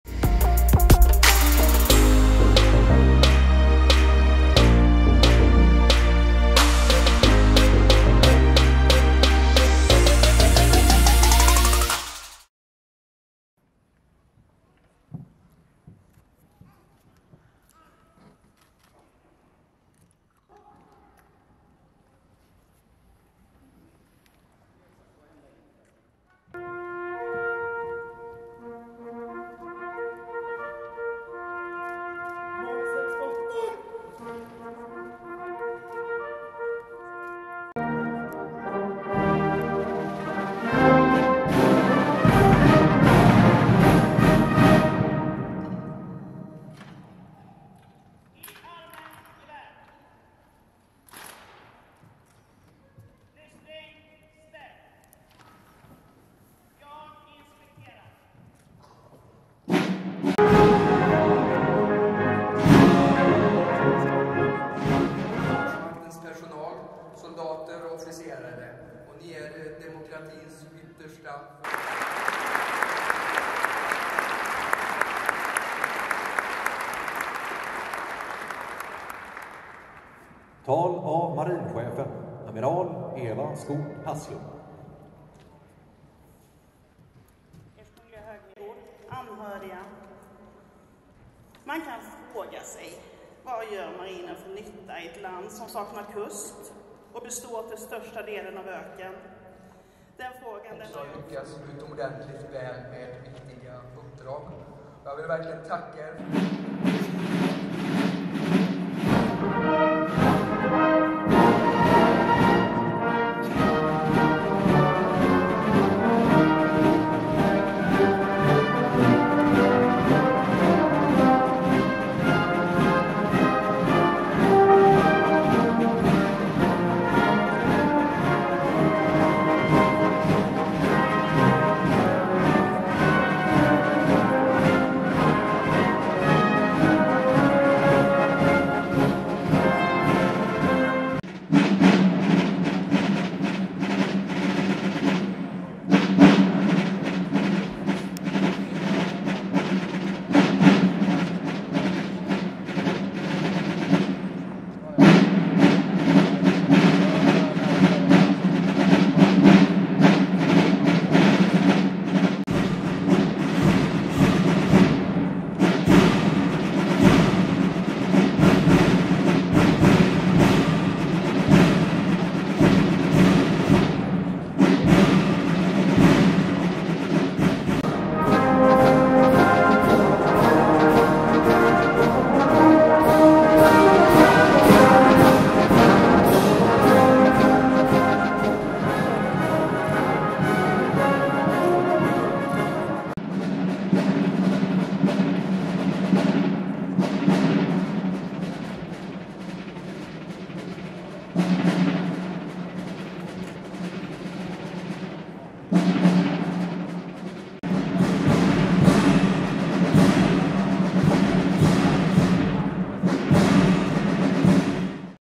Prince Carl Philip of Sweden at Swedish Army cermony
An new video when Prince Carl Philip of Sweden, in full military
clothes, participated at the medal ceremony for the Mali16 army unit which was distributed in the Royal Tennis Club in Stockholm. Speaches from army personel, music and medal cermony was on the agenda. Prince Carl Philip and Micael Byden, Supreme Commander of the Swedish Armed Forces, handed out medals.